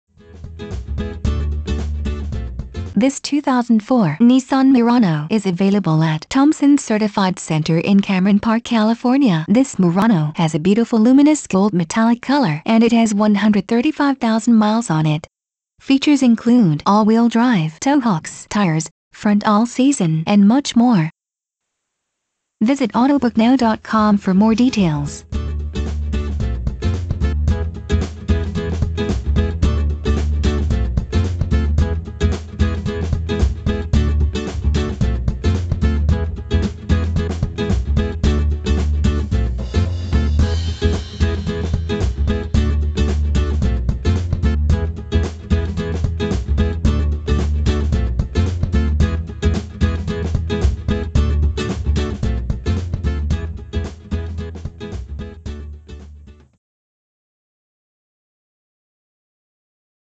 2004 Nissan Murano sl in Cameron Park CA for Sale
2004 Nissan Murano sl
Stock #: 34931A
Price: $10,988
Miles: 135,785
Trim: sl
Color: Luminous Gold Metallic
Interior: Tan SL Cloth
Transmission: Variable
Drivetrain: AWD